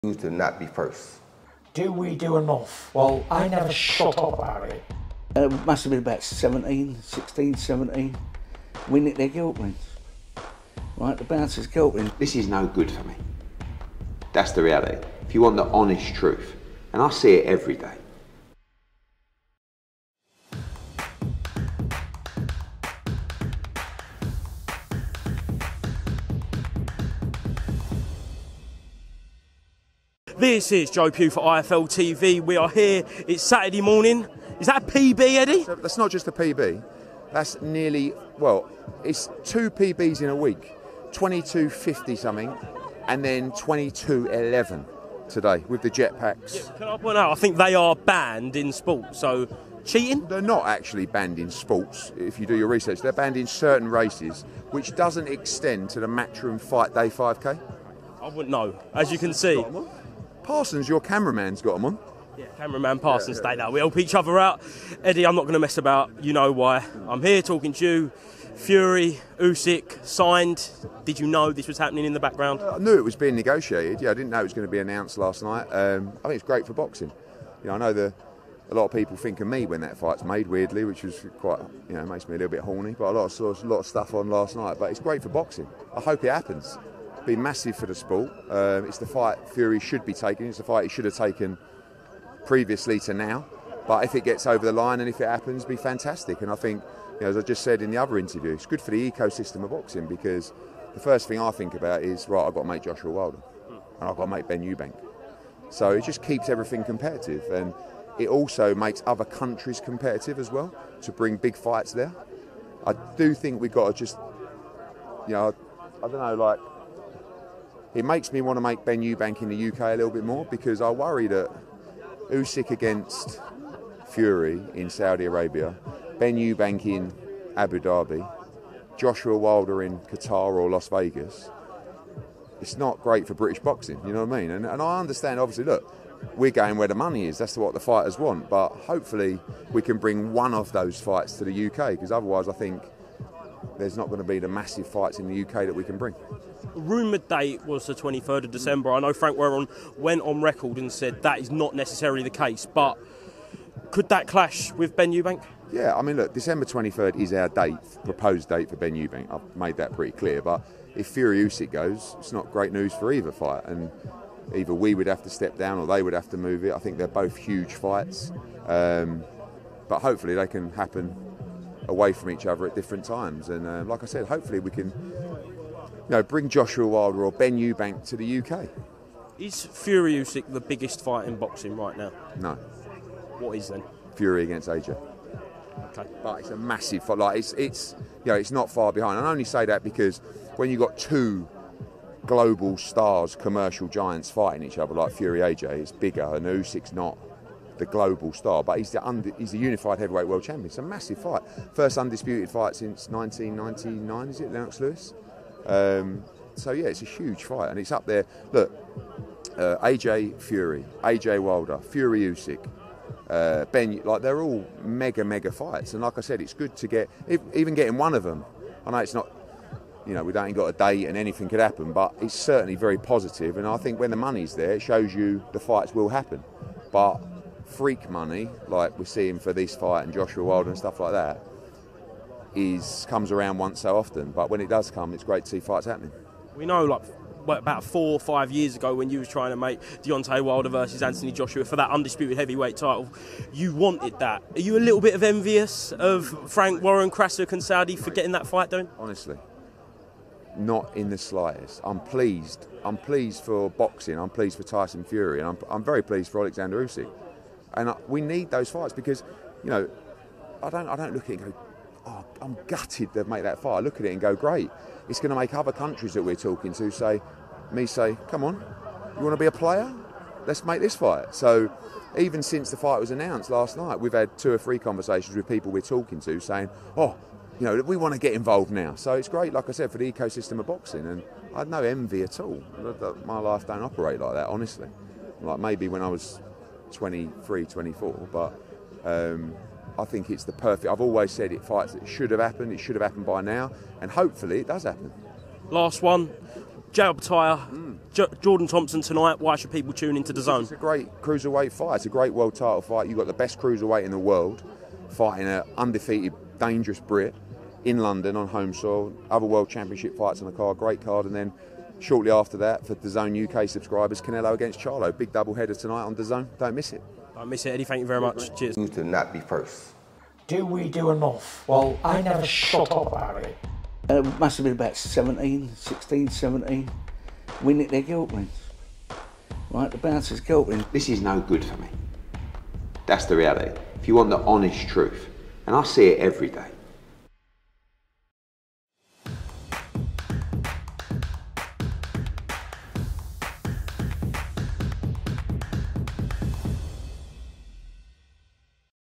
‘IT IS ******* MASSIVE!’ EDDIE HEARN REACTS TO FURY v USYK SIGNED, CLASHES WITH BENN v EUBANK JR
INTERVIEW BY @JOEPUGH_IFL